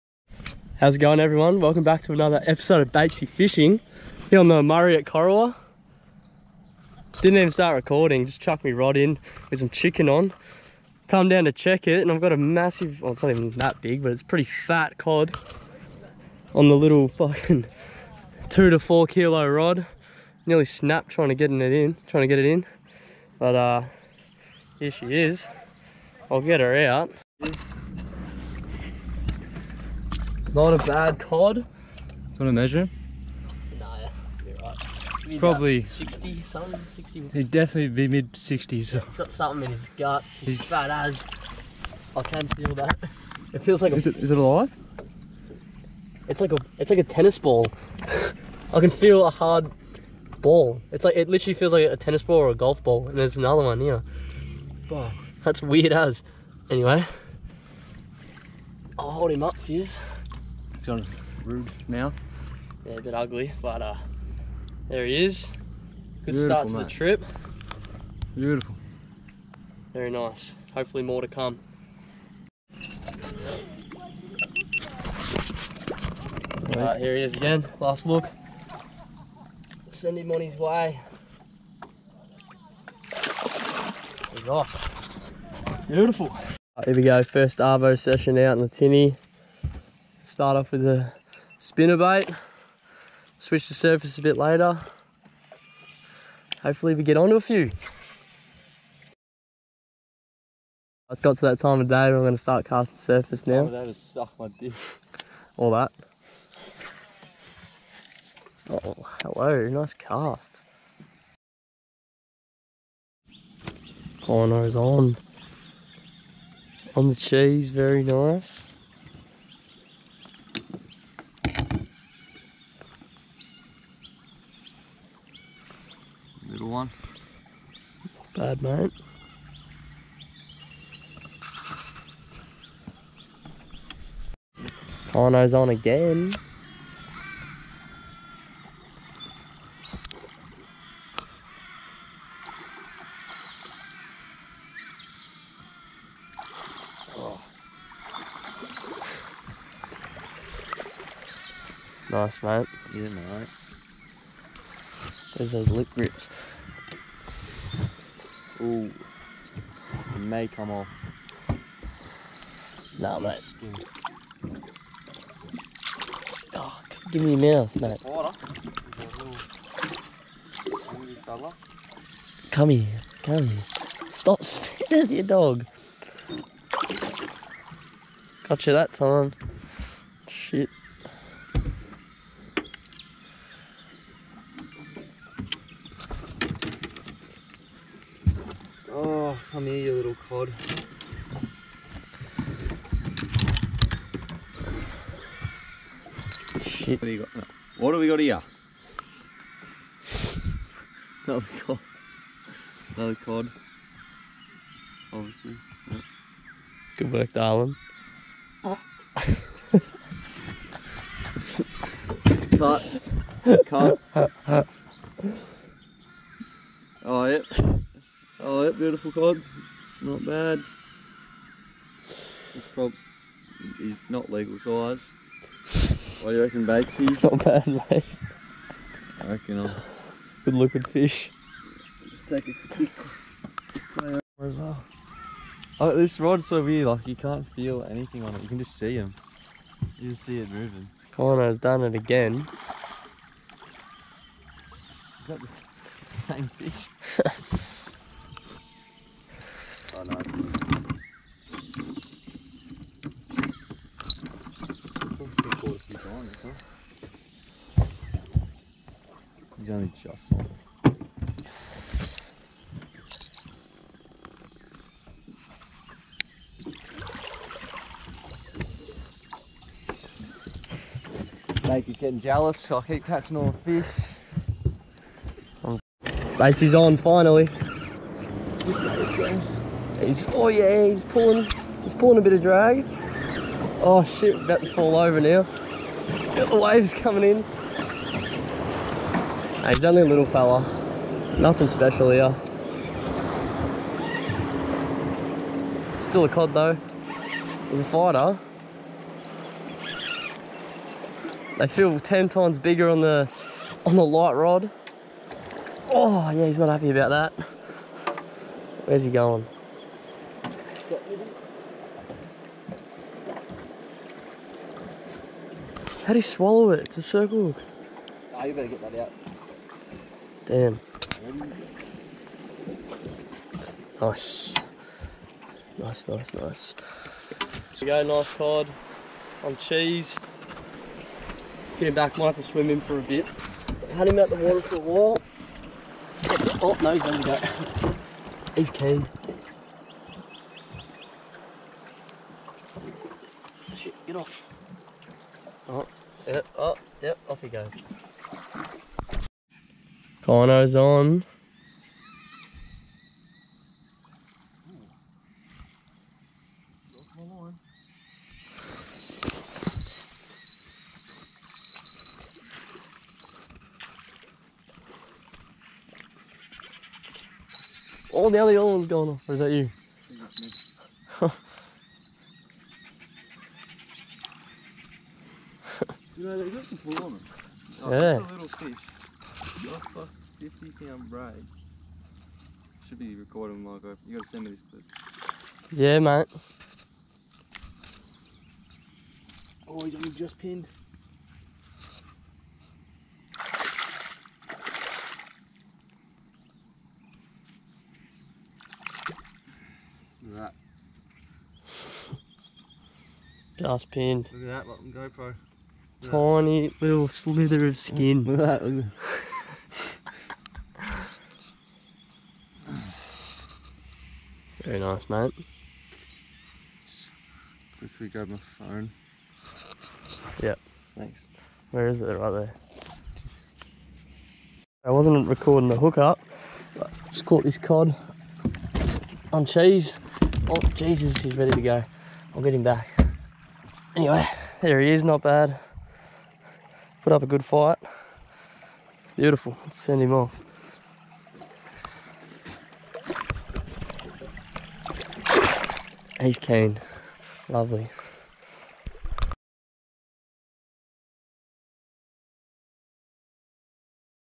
Fishing The Murray | Hunting The Metery: EP3
catching cod on the murray river with fishing with kyno